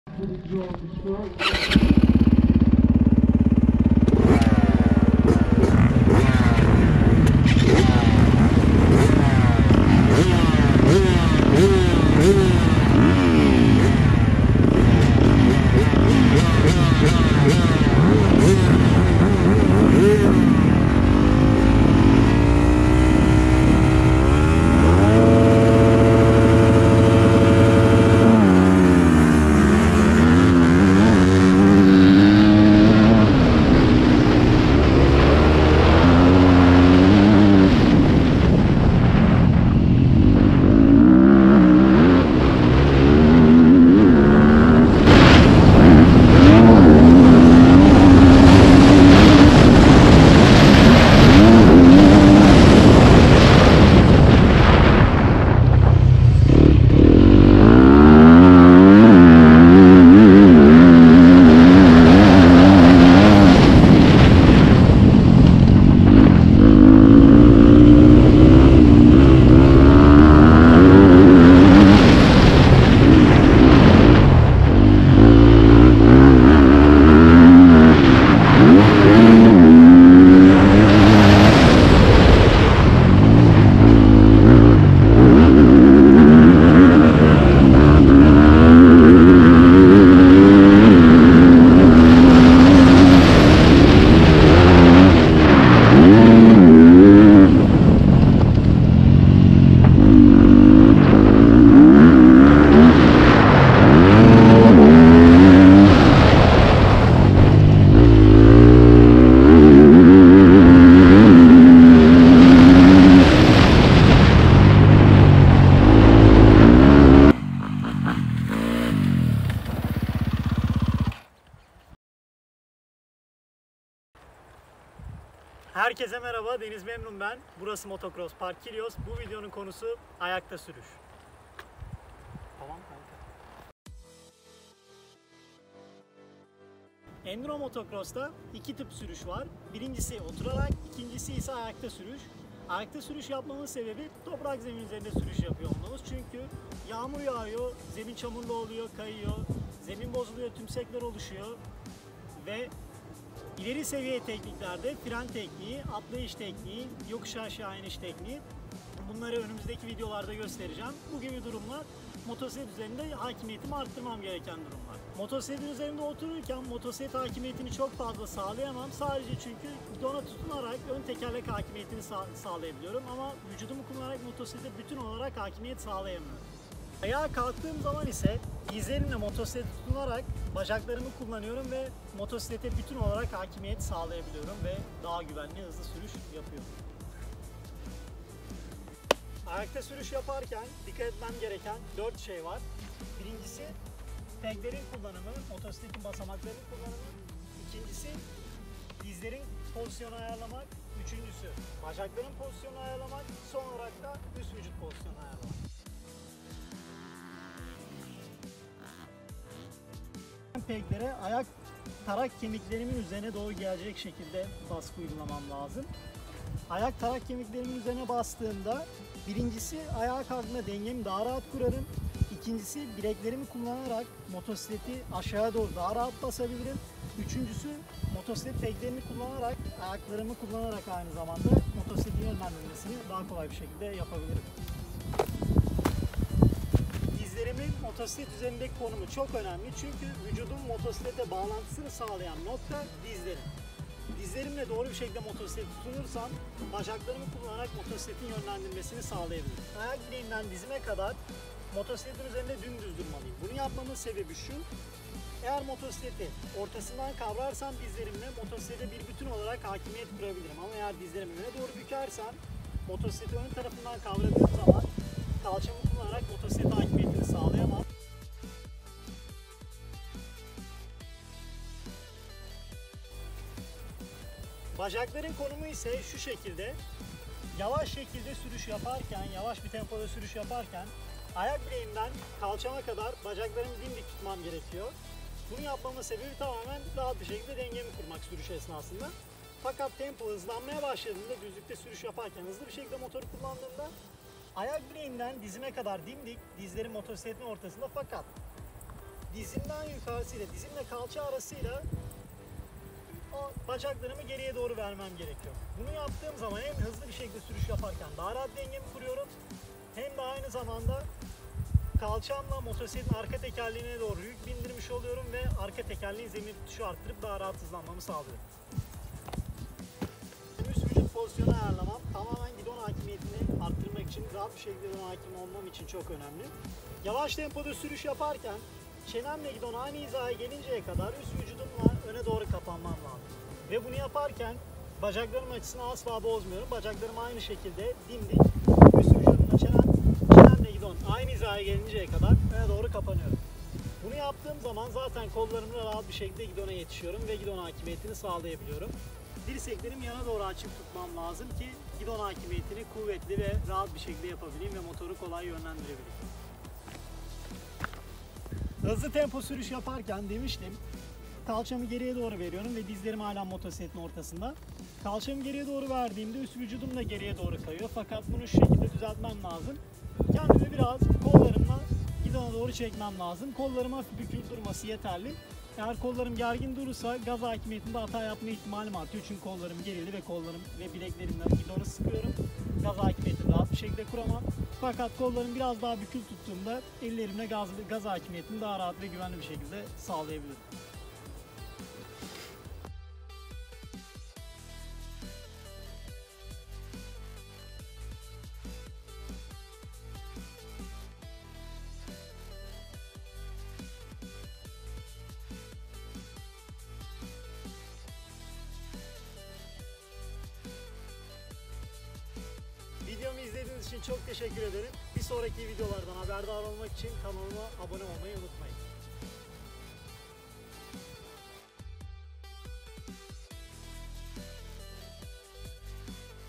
Enduro & Motokros Eğitimi | Ayakta Sürüş - Başlangıç
Doğru teknikler ile enduro & motokros aslında çok kolay! Bu videoda Deniz, size düzlüklerde ayakta sürüş tekniğini gösteriyor.